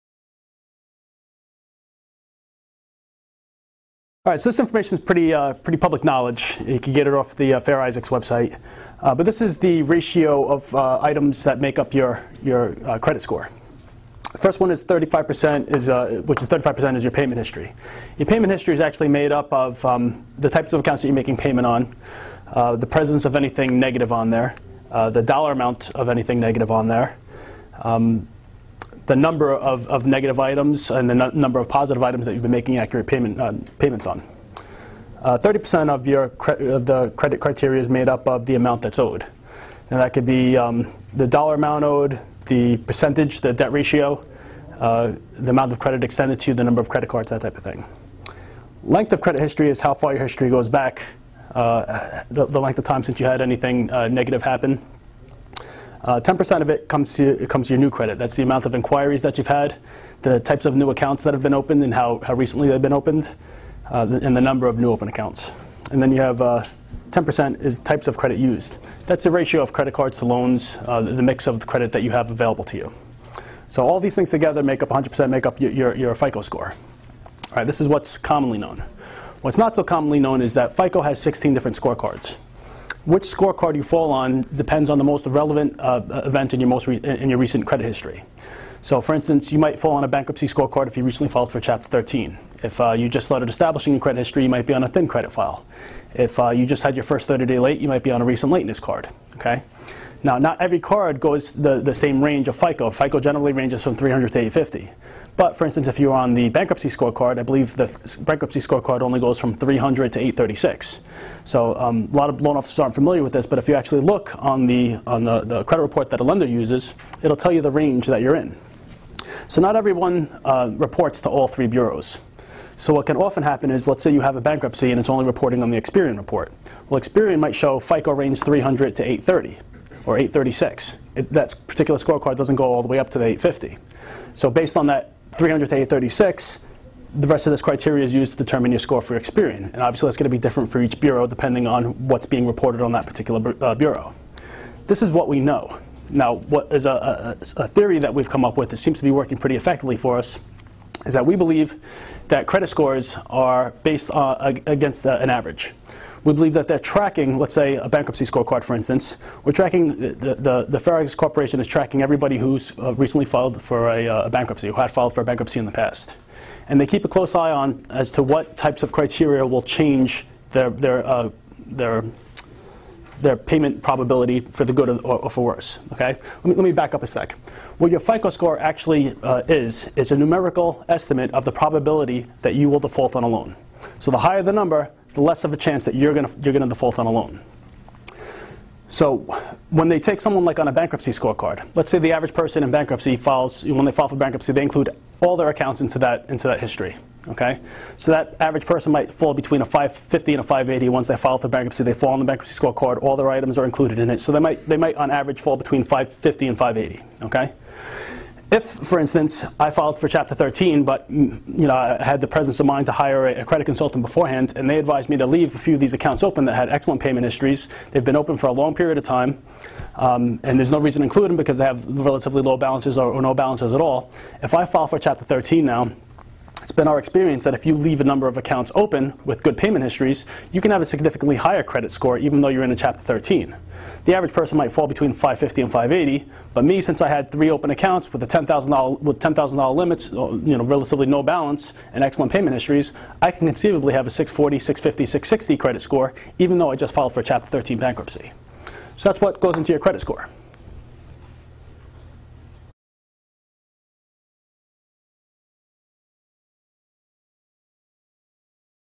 Credit Repair Exposed - The Truth About Credit Scores
- Credit expert, Brian Diez, reveals what the bureaus hope you'll never learn about your credit scores.